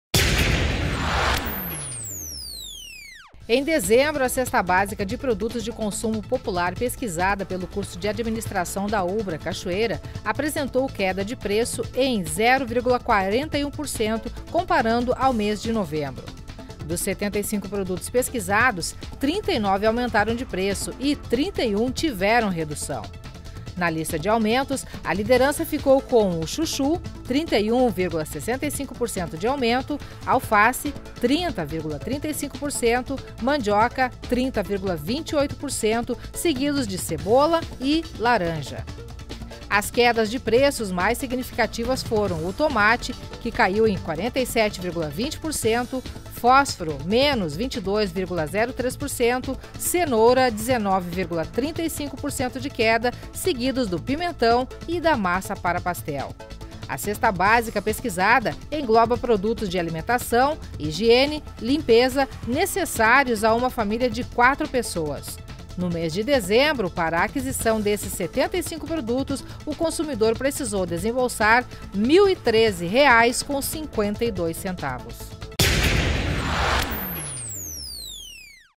Cesto Ulbra cai 0,41% em dezembro
Tomate foi o campeão em baixa com redução de preço em 47,20%